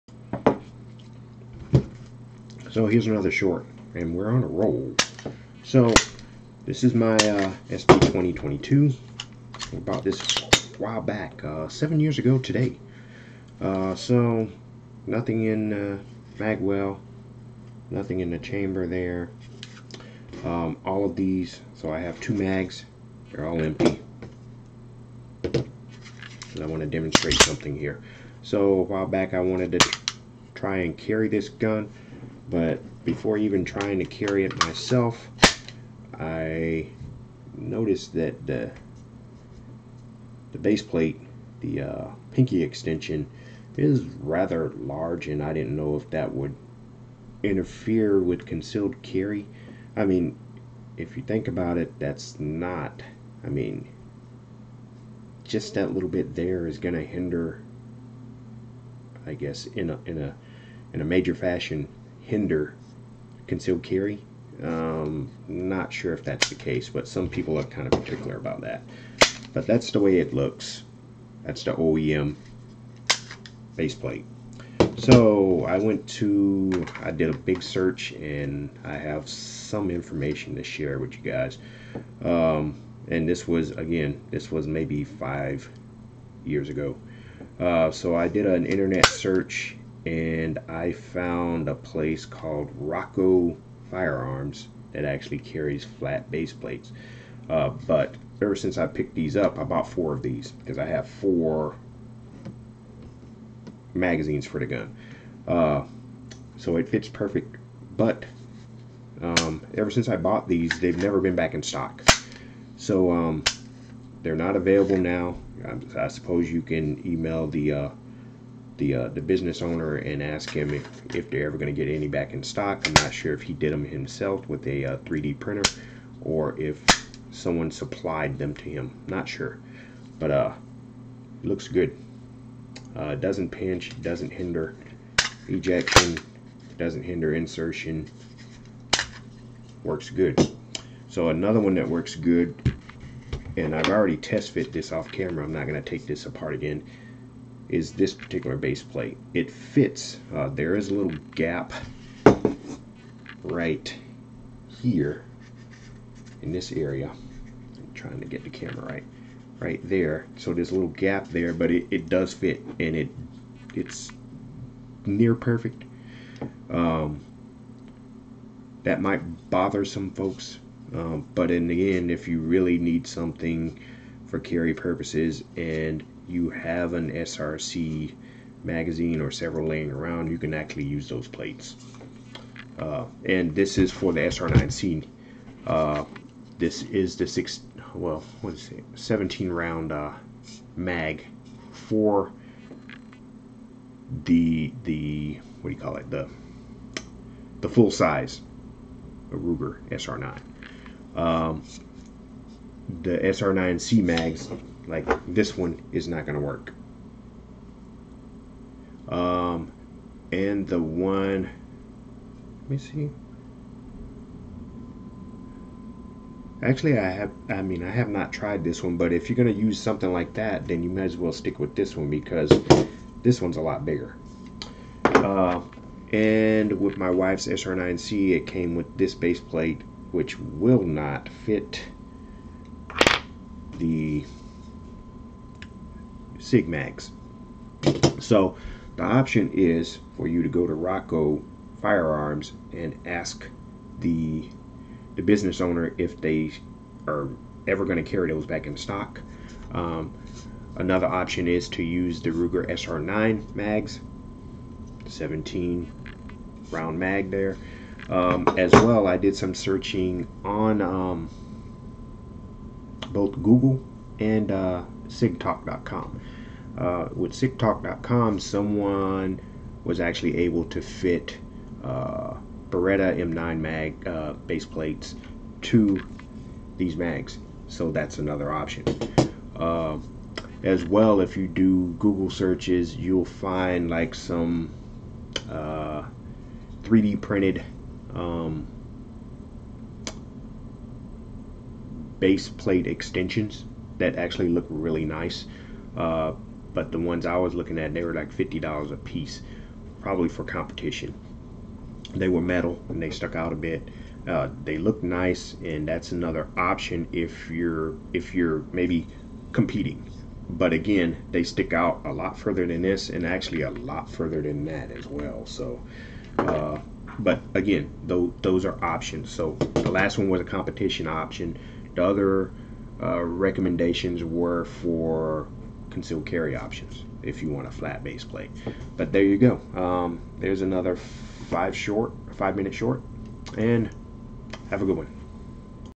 Gun Shorts - SP2022 Mag Base Plate Options
This is part of my 5 min video segment.

I've done some research for those folks that conceal-carry the SP2022.

In fact, I talk of SP2022 mag base plate options for both CC and competition.